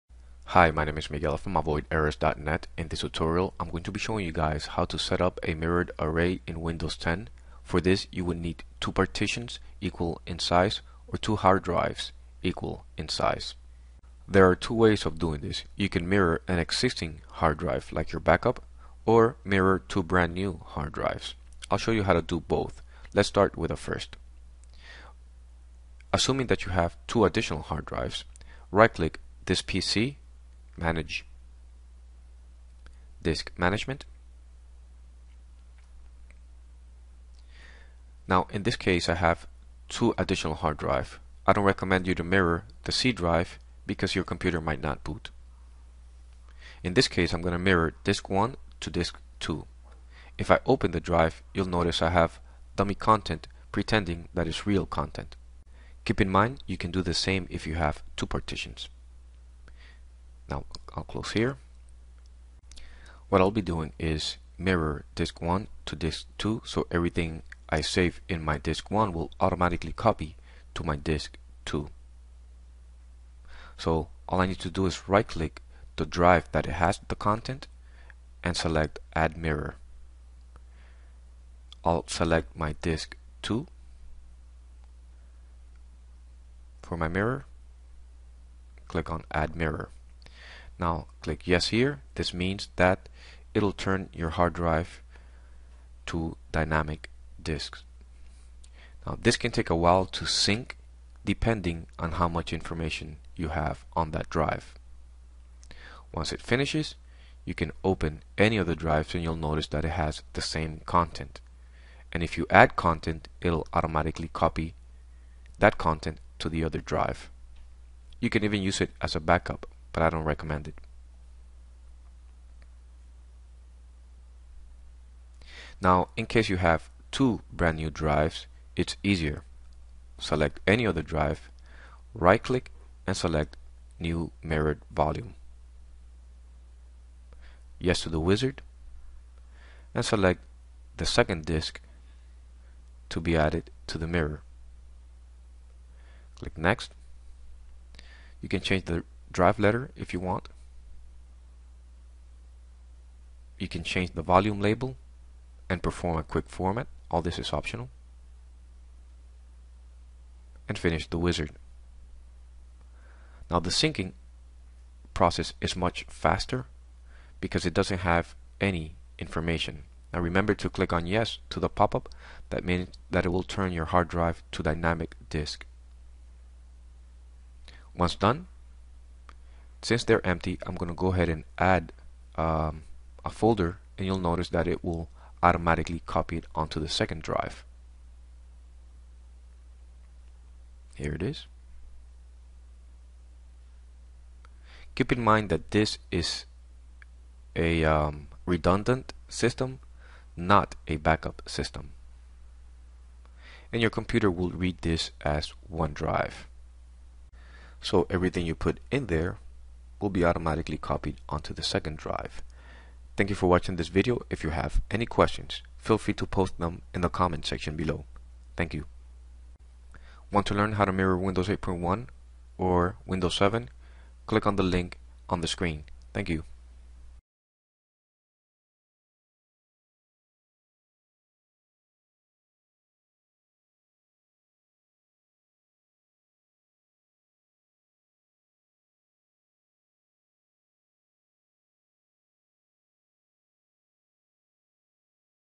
How to Set up a Mirrored Array in Windows 10
In a mirrored array, all of the data is duplicated across 2 or more hard drives. Windows 10 mirroring is only available in Professional edition